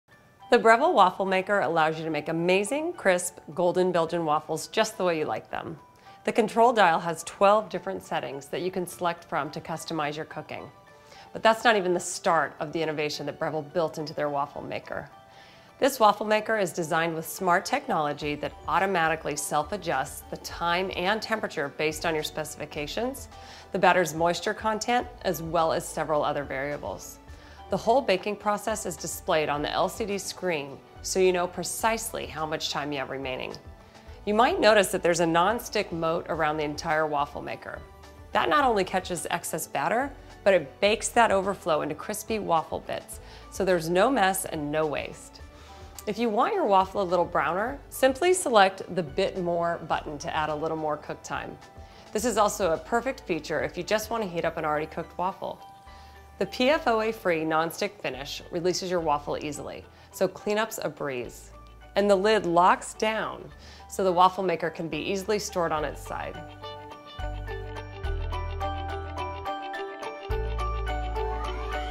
Get to Know the Breville Smart Waffle Maker | Williams-Sonoma
Say "good morning" to an innovative machine that lets you make Belgian-style waffles exactly the way you like them—with more options for precise customization than any other waffle maker on the market. Breville's smart sensor technology automatically adjusts the temperature and baking time to guarantee perfect, personalized results. A nonstick moat prevents overflowing batter from making a mess on the counter.

The waffle maker featured in this video can be purchased from Williams-Sonoma